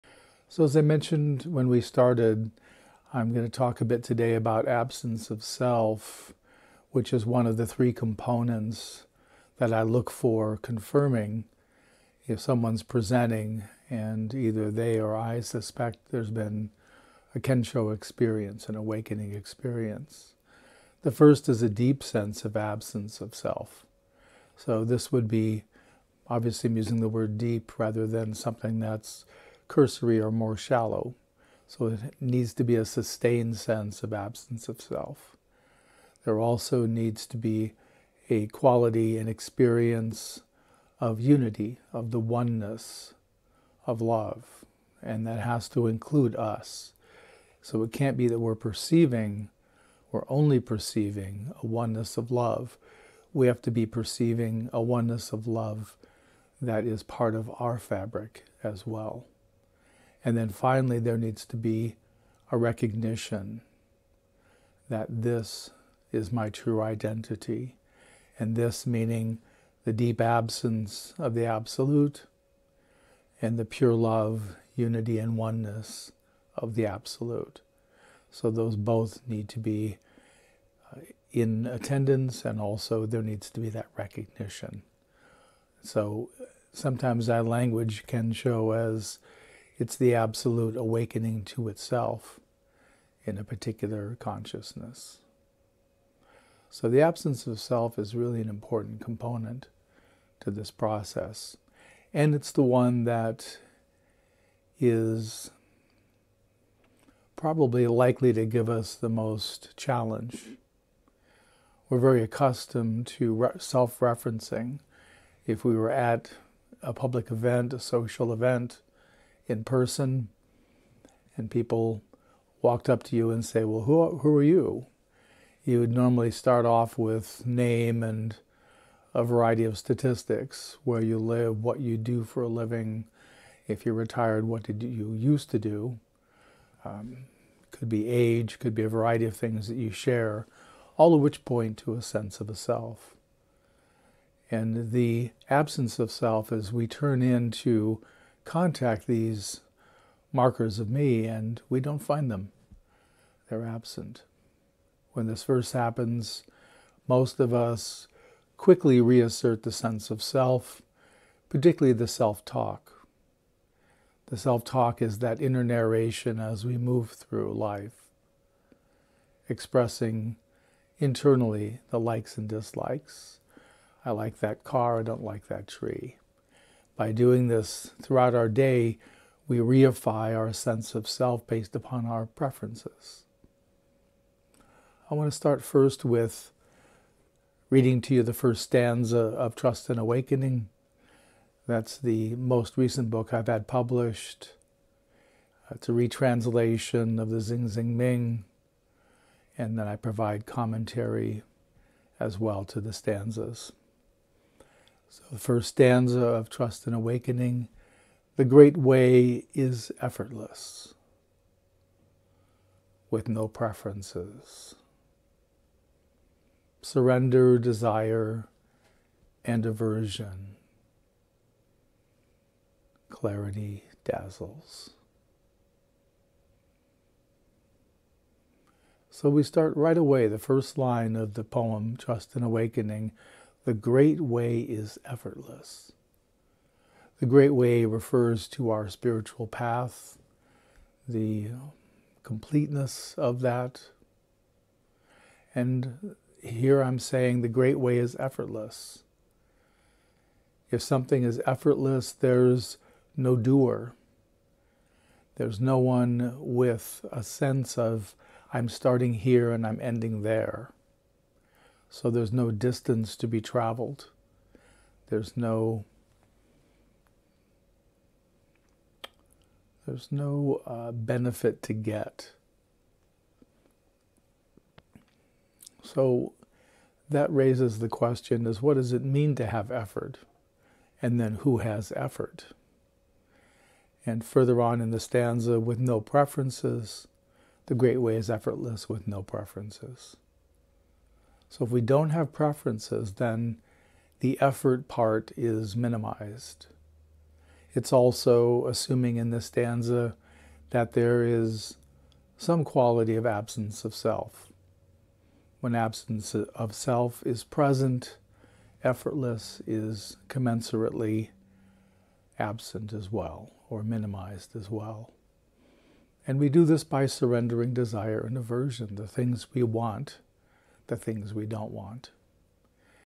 Absence of Self - No Effort, No Preferences
This is an excerpt from a Dharma talk Stephen gave to his Demystifying Awakening Mentoring Group - Dec 9, 2023